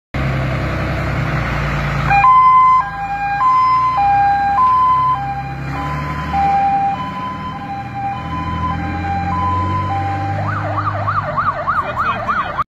Alarm 10 05 2021 godz. 21 34 Pożar budynku gospodarczego w Czarkowie
Zacznij wspierać ten kanał, a dostaniesz te bonusy:
Polecam ten portal wszystkim i zapraszam do polubienia strony
Pozdrawiam serdecznie